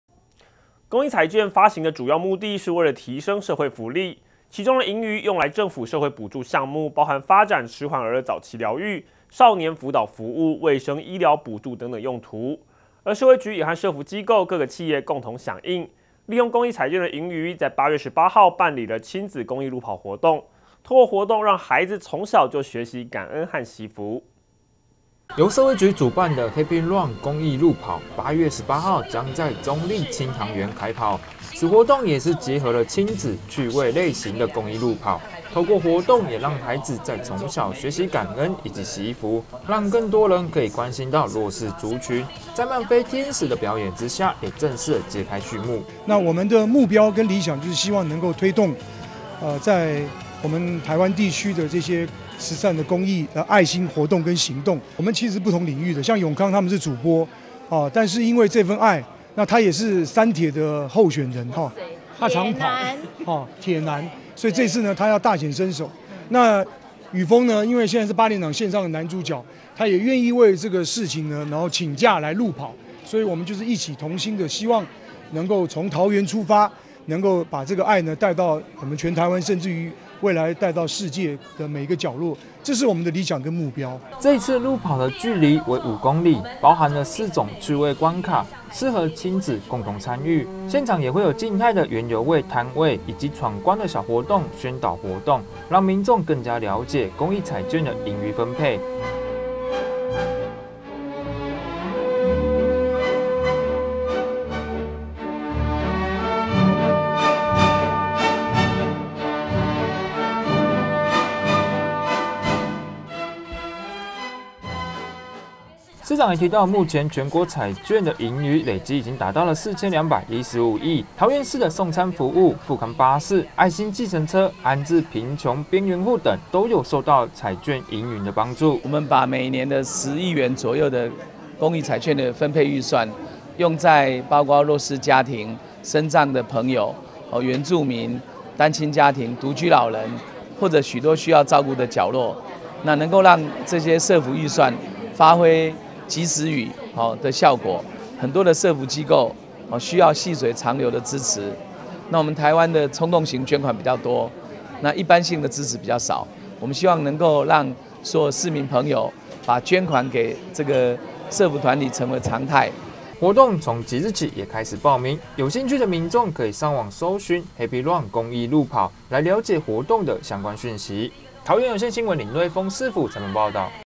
桃園有線新聞20180619-818公益路跑賽 跑出健康助弱勢
公益彩券發行的主要目的為提升社會福利，其中的盈餘用來政府社會補助項目，包含

發展遲緩兒童早期療育、少年輔導服務、衛生醫療補助等用途，社會局也與社福機構、各企業共同響應，在8月18號舉辦親子公益路跑活動，透過活動讓孩子從小學習感恩及惜福。

市府採訪

由社會局主辦的Happy Run公益路跑8月18日將於中壢青塘園開跑，此活動也是結合親子、趣味、類型的公益路跑，透過活動也讓孩子從小學習感恩及惜福，也讓更多人可以更關心到弱勢族群，在慢飛天使的孩子表演下，也正式揭開序幕。

台灣藝起發光促進協會理事長宋逸民

這次路跑距離為五公里，包含四種趣味關卡，適合親子共同參與，現場也會有靜態園遊會攤位，以及闖關小活動、宣導活動、讓民眾更了解公益彩券盈餘分配

市長也提到，目前全國彩券盈餘累計已達4,215億餘元，桃園市送餐服務、復康巴士、愛心計程車、安置貧窮邊緣戶等，都有受到彩券盈餘的幫助

市長鄭文燦

活動即日起也開始報名，有興趣的民眾可以上網搜尋Happy Run公益路跑，來了解相關的活動訊息